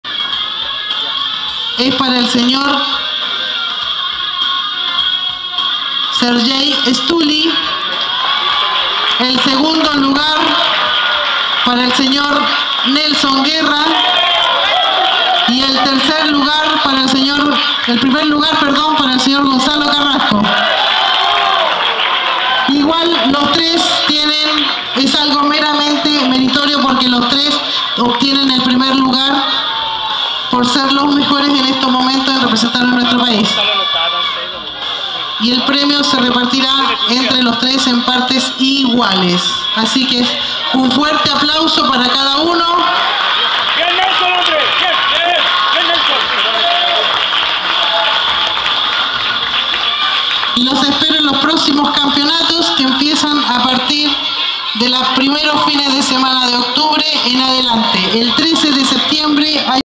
Gonzalo Carrasco overall winner 2009
Premiación 1º Gonzalo Carrasco, 2º Nelson Guerra, 3º Sergey Stuli - Selectivo Sudamericano Lima 2009